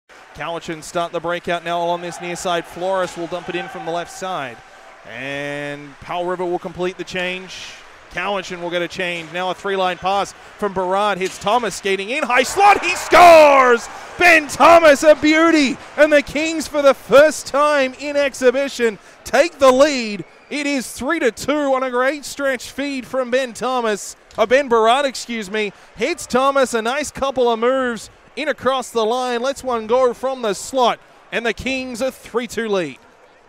Ben Thomas' Snipe vs Cowichan
Ben Thomas finds himself on the receiving end of a Ben Berard stretch pass and finishes for a big second period goal against the Cowichan Valley Capitals on August 25th, 2018